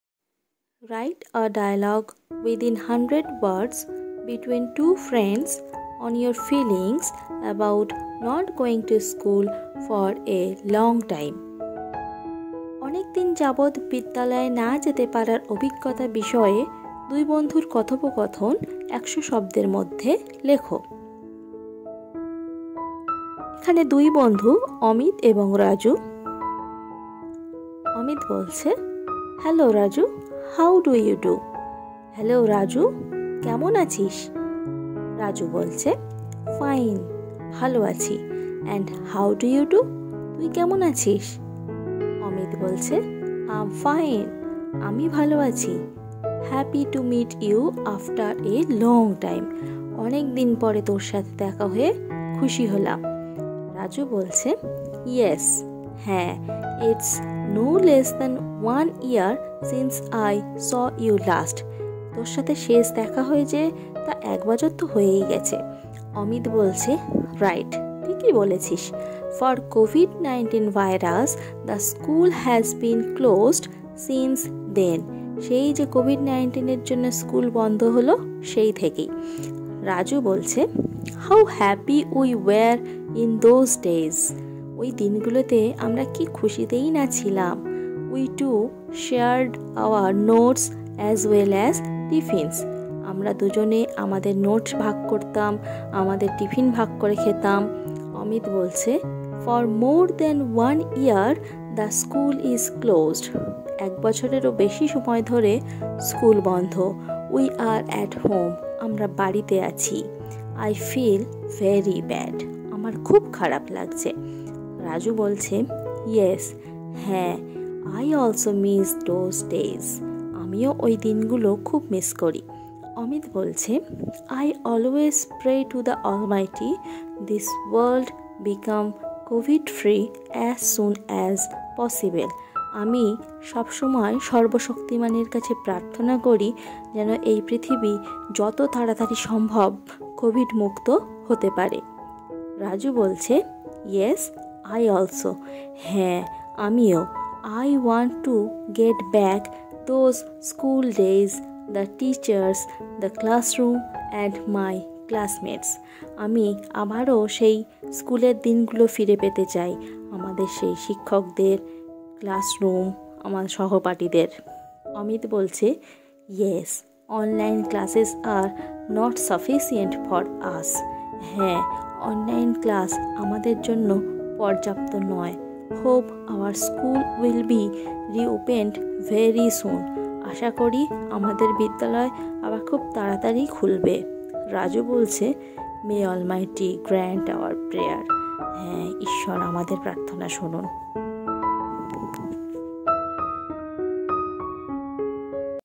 dialogue between two friends on your feelings about not going to school for a long time
Write a dialogue between two friends on your feelings about not going to school for a long time .

Writings, Paragraph, Letter ,Story, Notice,Processing, Report

Model activity task October-November(Class 1 to class 10) all Activity Tasks